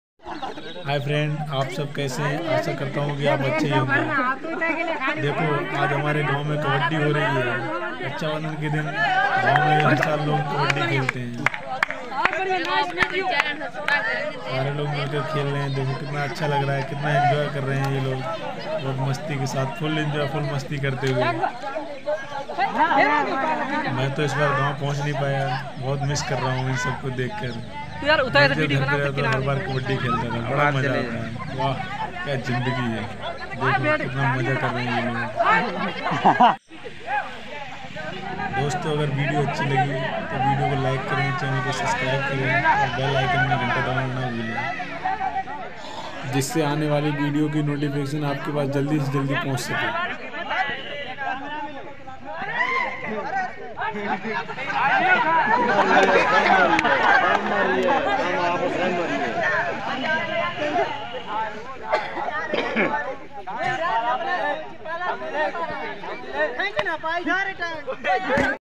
Kabaddi 🙃 Match of village boys | ganv ke ldko ka Desi Kabadi match vlog
🙂Kabaddi 🙃 Match of village boys | ganv ke ldko k ladko  Desi  Kabadi  match |unbelievable  game
carzy barsha.

kis kis ne ye chhote par khela h jo abhi tk miss krte h